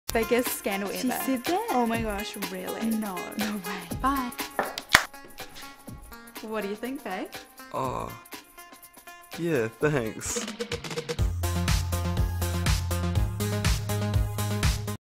New U Channel Indent
Watch the New U Channel Indent featuring Luke Whyle and Samantha Nicholas in featured roles, Luke even got a pedicure for his role, that's dedication!